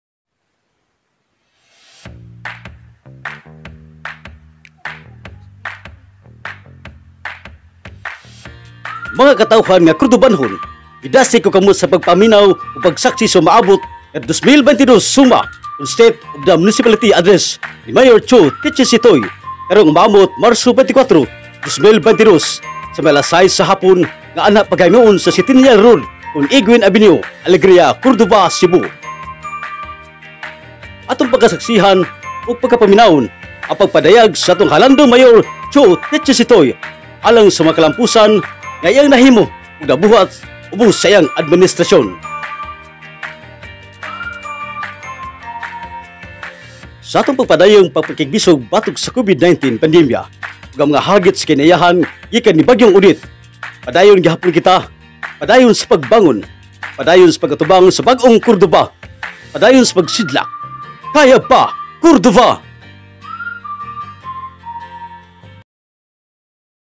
SOMA 2022 Teaser | Municipality of Cordova
WATCH: 𝐒𝐎𝐌𝐀 𝟐𝟎𝟐𝟐 Teaser Video
State of the Municipality Address karong Marso 24, 2022, alas 6 sa gabii, sa Centennial Road.

Edited by: CLE ANN CARO TUÑACAO, CORDOVA PIO
Voice-over: BRENDO SUMAGANG, ENGINEERING OFFICE
Arranged by: GERRA PADILLO PALSARIO, MUNICIPAL ADMINISTRATOR

Drone shots/Vidoes/Photos by (CPC Intern):
John P. Alfafara Jr.
Jane Claude C. Espra
Johnmar Trazona
Harold Archival